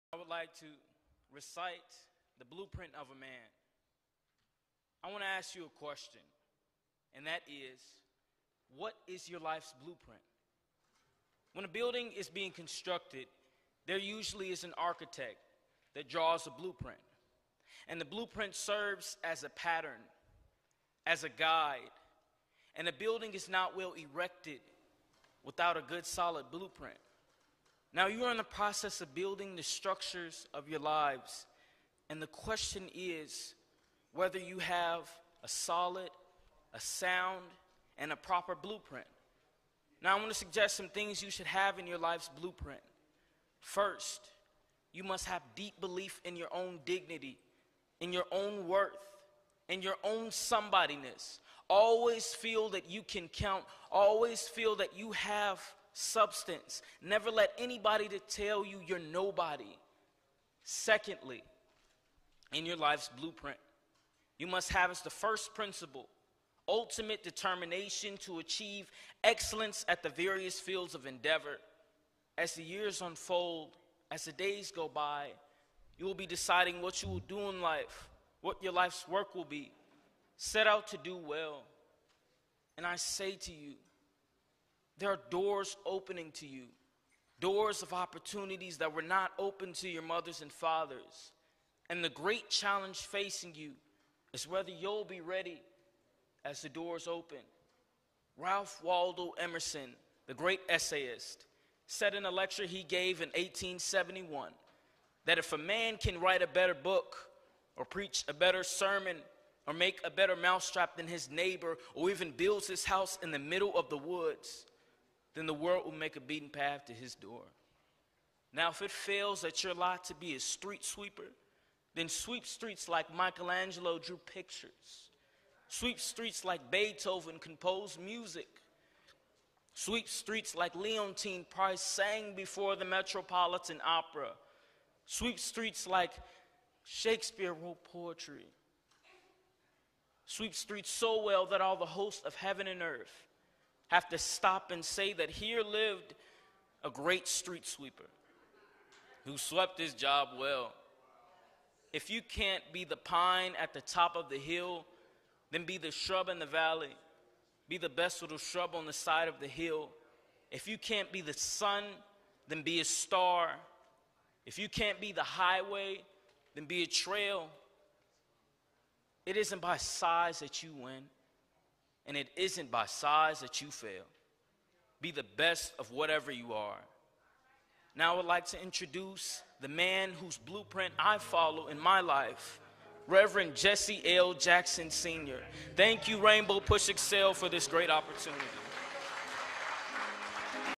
Oratorical student
Oratorical student speaking during the Rainbow PUSH Coalition Saturday Morning Forum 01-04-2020 at Parkway Ballroom Chicago, IL